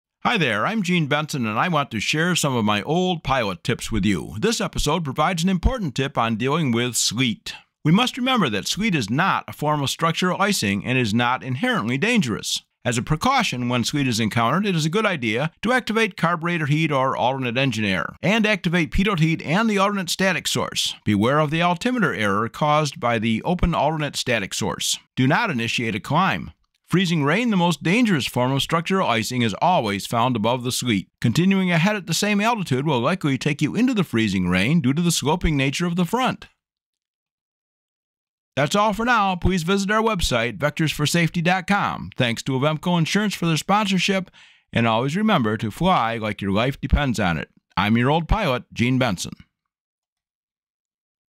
OPT-16 Dealing with Sleet
View this video to learn some valuable tips about what to do and what not to do if sleet is encountered during a flight.